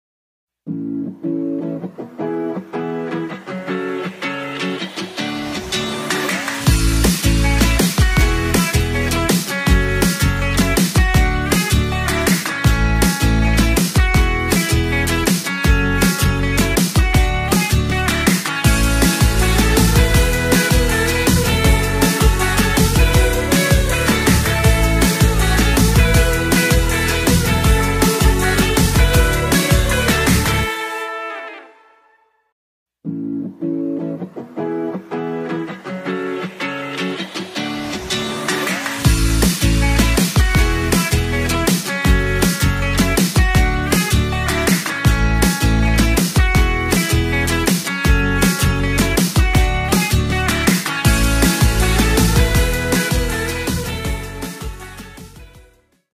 CABLE KICKBACKS by 4 X Women's World Bodybuilding Champion - Lilian Tan
Another variation to hit the glutes!
Cable Kickbacks, for that toned and shapely glutes everyone wants.